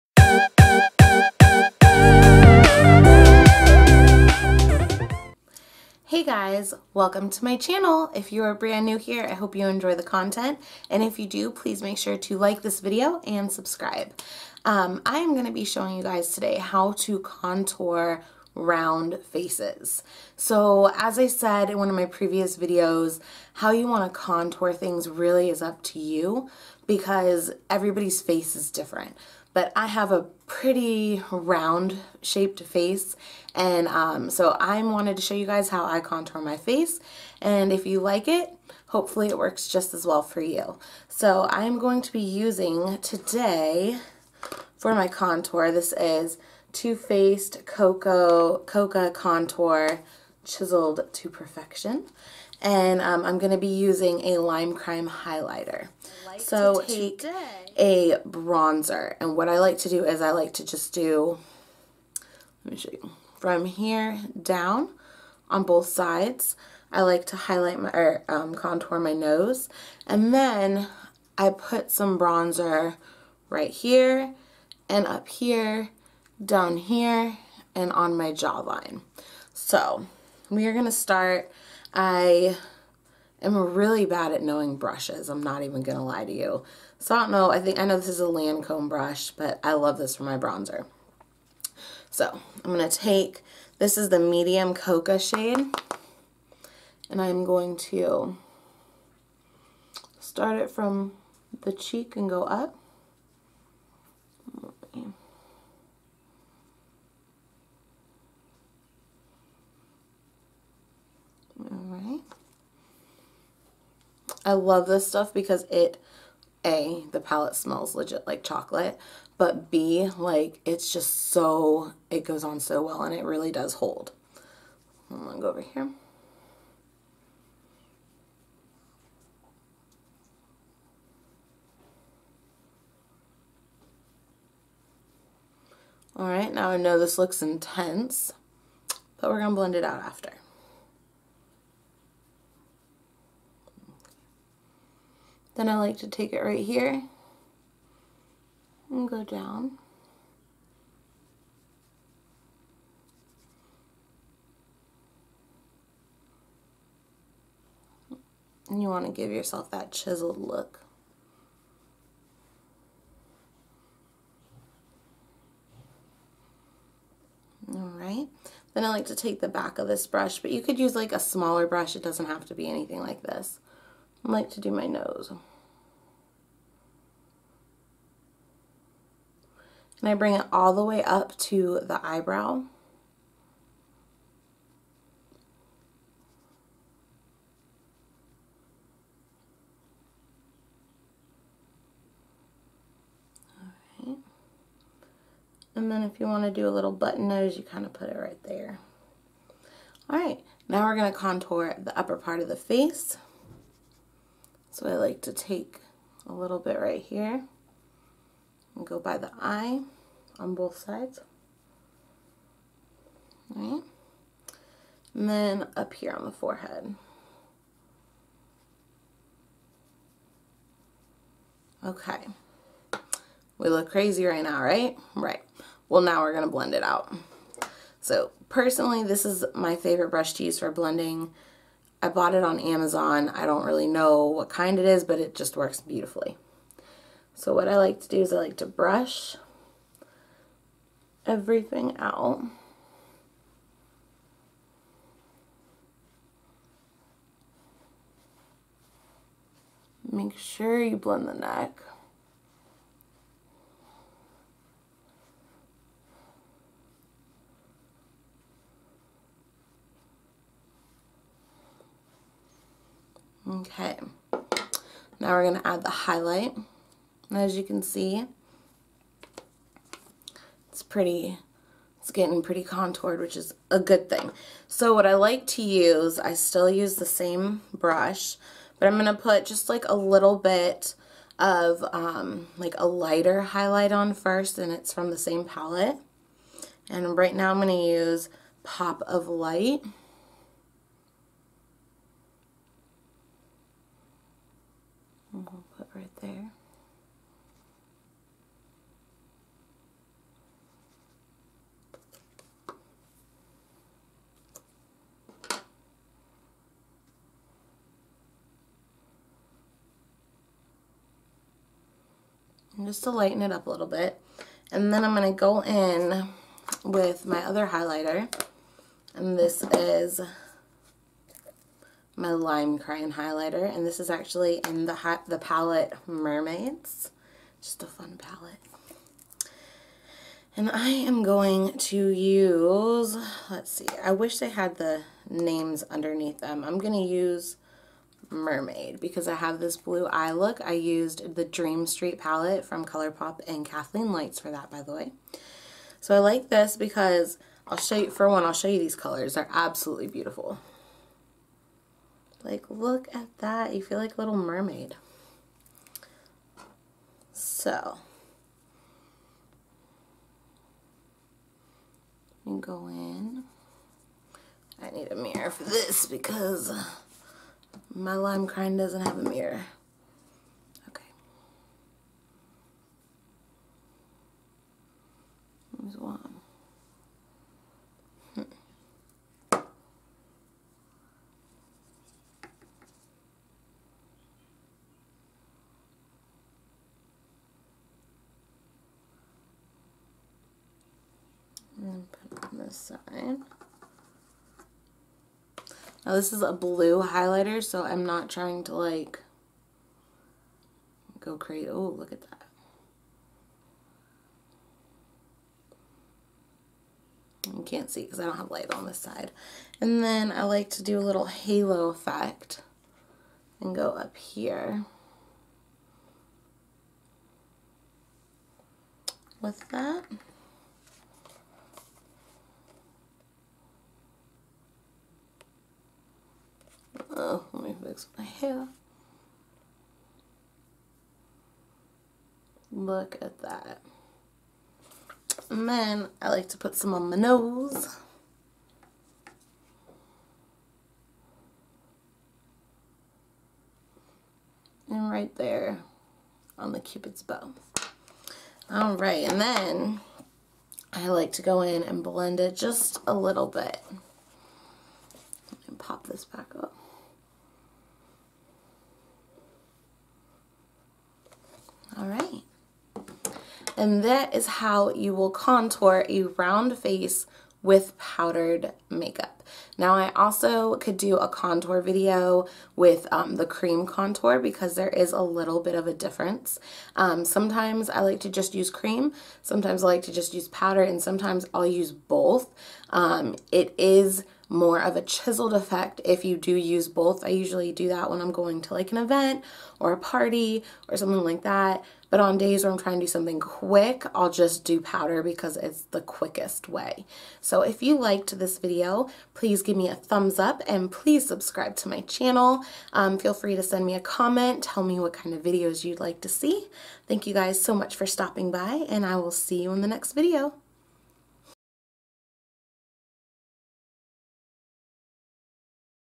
How to contour a round face
Ever wonder how to contour a round face? Everyone has different face shapes, so not every contour video will work for you. This is a video that will show people with round faces how I contour my round face. If you like it, let me know how these tips work for you!